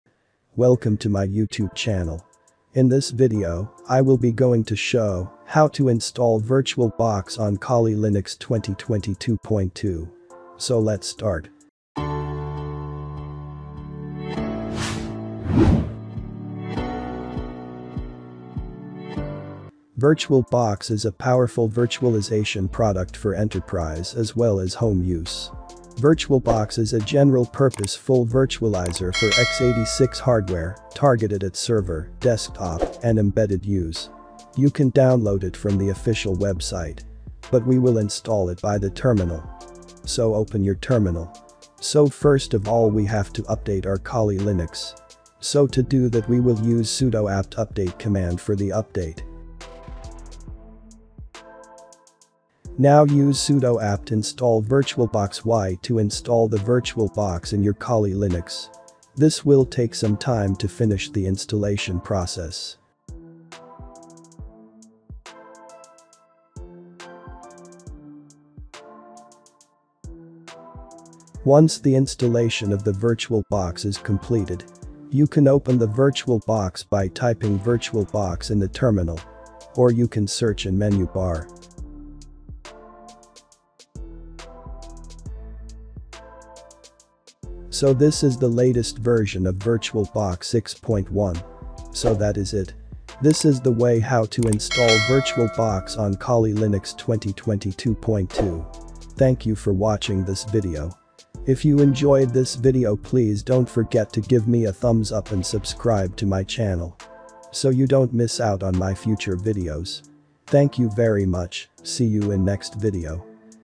How to Install VirtualBox in Kali Linux | Kali Linux 2022.2
In this video I will show How To install VirtualBox in Kali Linux 2022.2. Virtual Box is a powerful virtualization product for enterprise as well as home use. VirtualBox is a general-purpose full virtualizer for x 86 hardware, targeted at server, desktop, and embedded use. You can download it from the official website.

🕘 Timestamps:
[00:00] ▶️  Welcome
[00:11] ▶️  Intro
[00:41] ▶️  Update Kali Linux 2022.2
[00:53] ▶️  Install the VirtualBox
[01:20] ▶️  Run the VirtualBox
[01:44] ▶️  End

That is it! This is the way How to Install VirtualBox in Kali Linux 2022.2.